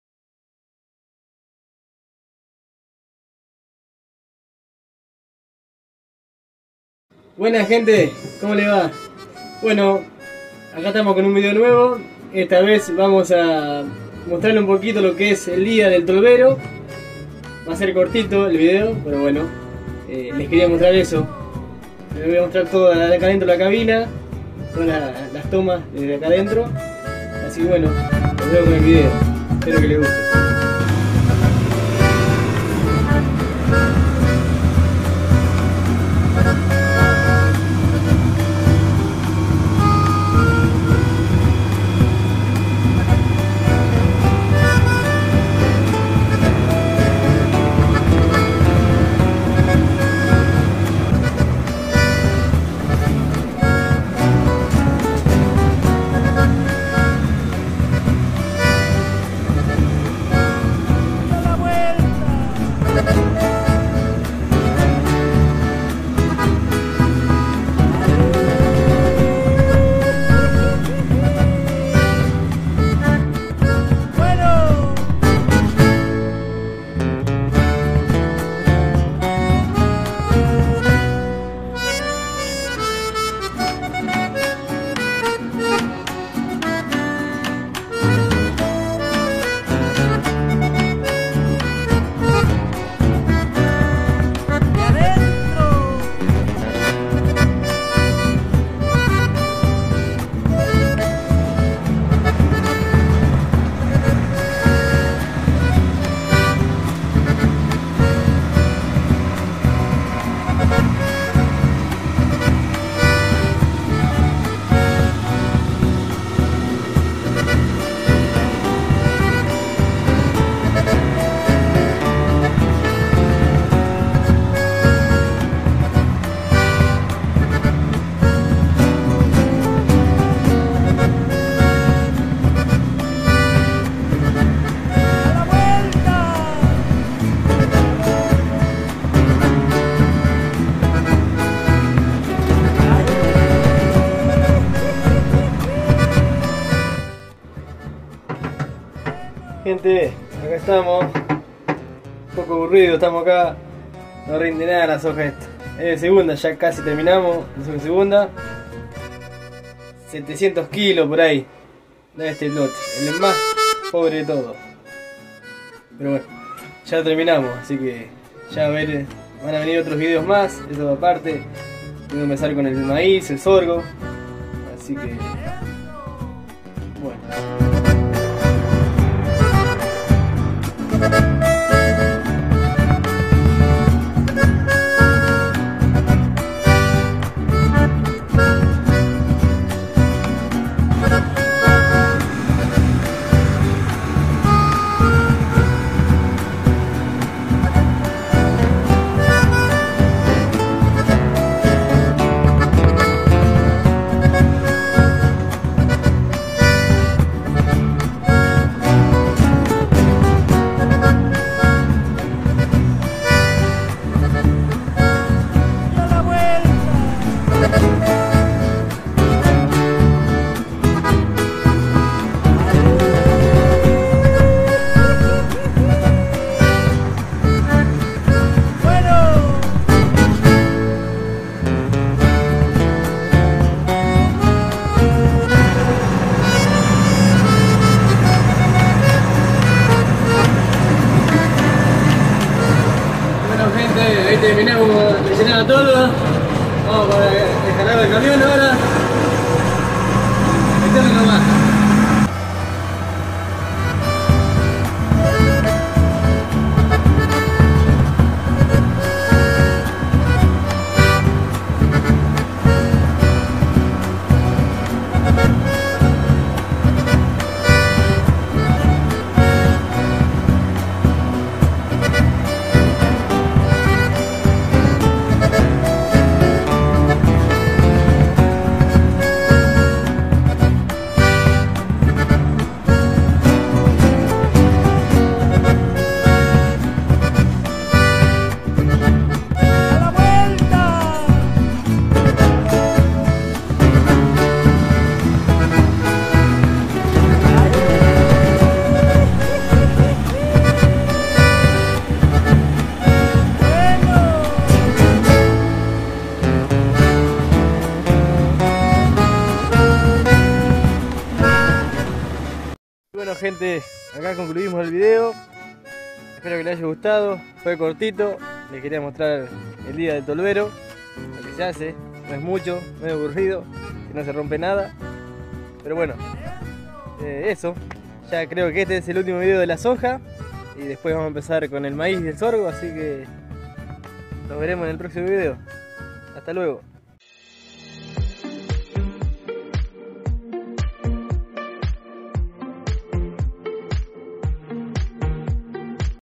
La labor del tolvero en la cosecha de soja Argentina | FCA
Buenas gente, en este video les mostraré el labor del tolvero en la cosecha de Soja de segunda en Argentina, espero que les guste y ya proximo estaremos con el maíz y sorgo!